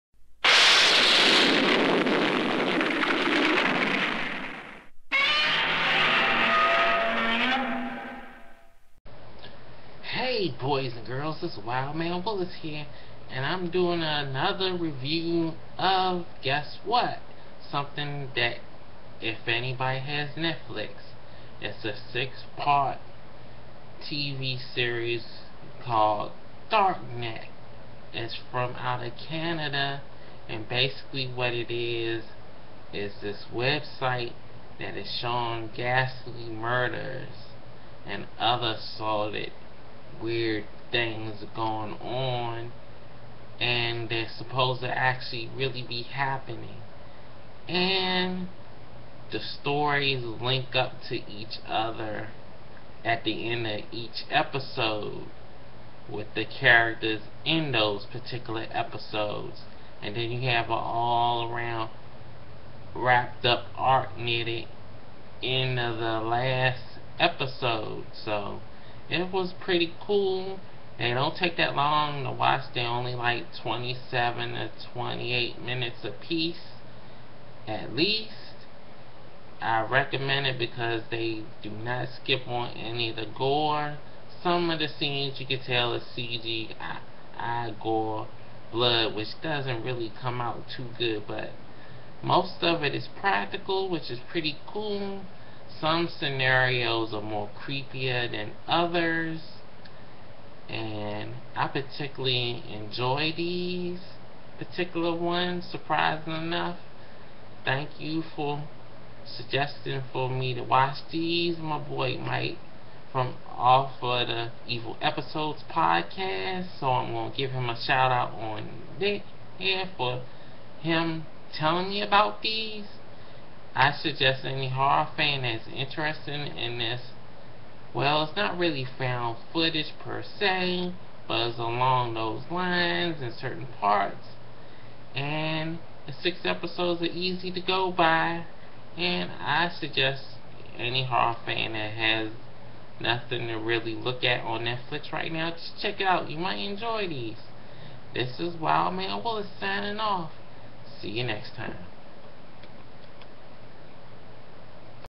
wild man willis review of darknet on netflix
my look at the six parts of darknet